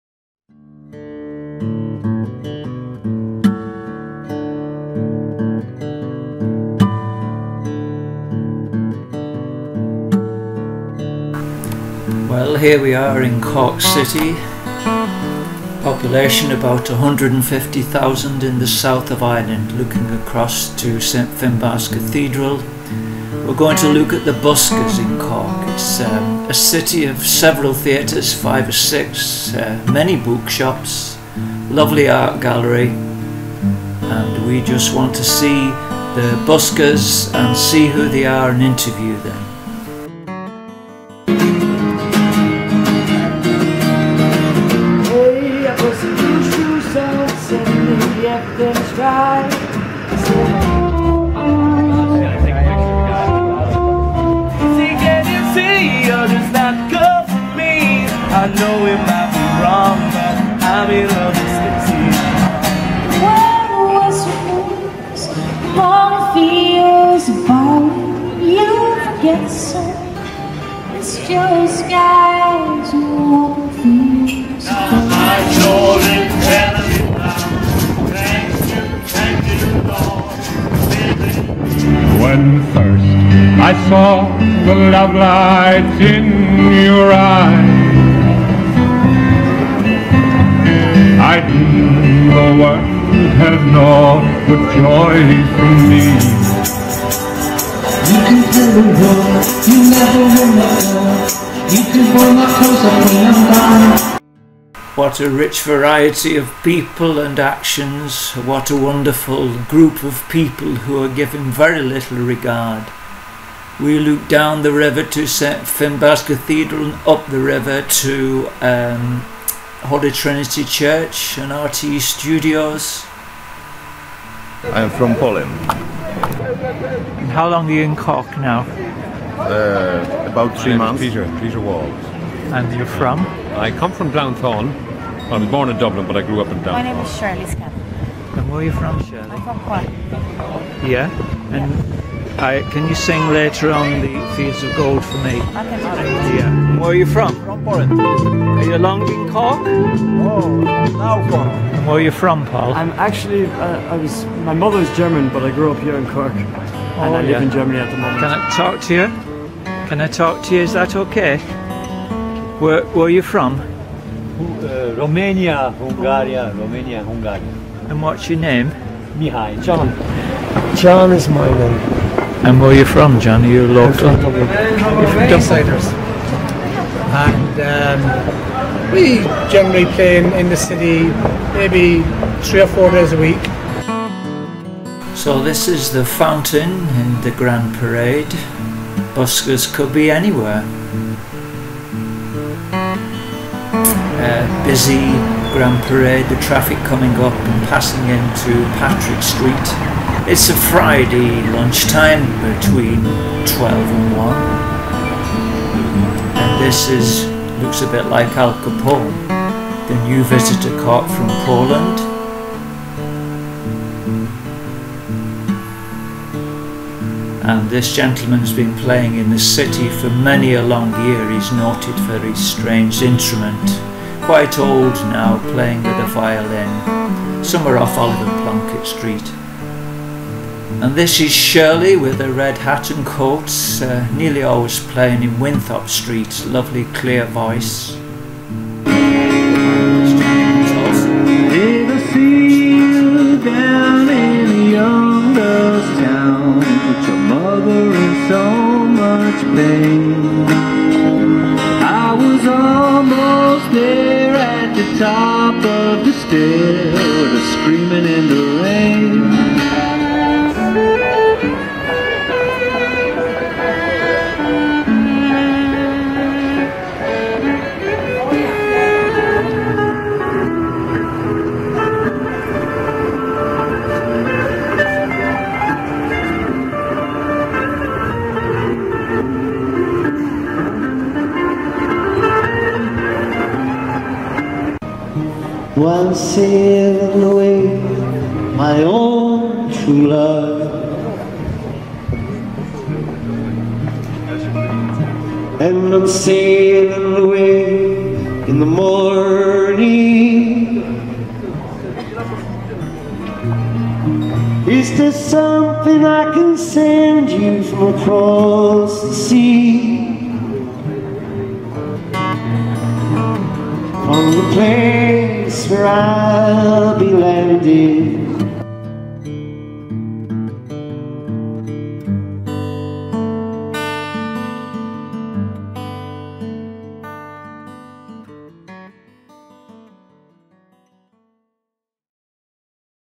CORK MAKES MUSIC!..... a compilation of Cork buskers
brief snatches of buskers making music on the streets of Cork in the first few months of 2013. brief interviews with them. where they are from..,,,..their songs. really nice guys! The streets of Cork are alive with music. Cork makes music!!!